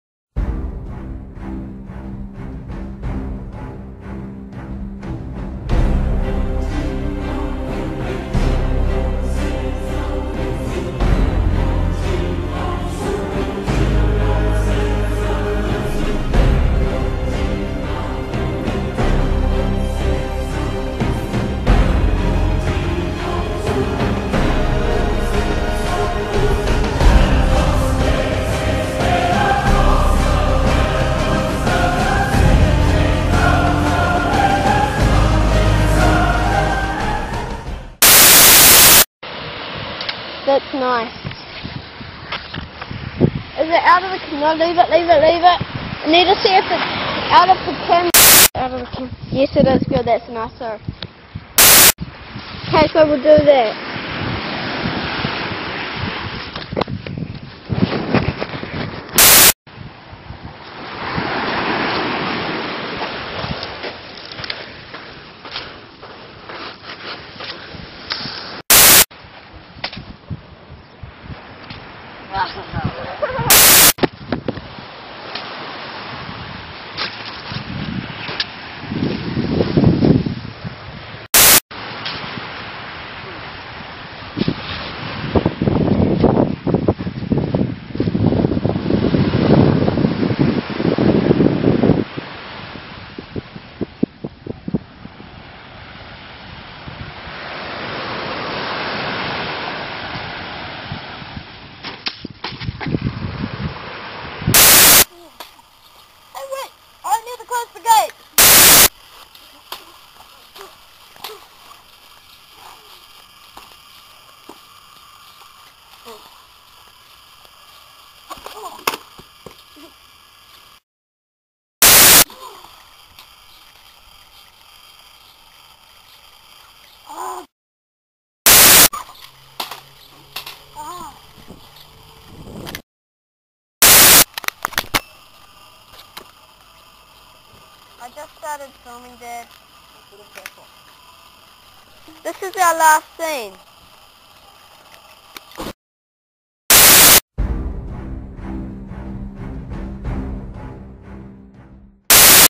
The Ruined Crucible 4 Bloopers
Darth James and Liam Wan Kenobi II fool around behind the scenes of The Ruined Crucible 4: Revenge of the Jedi. Errors galore!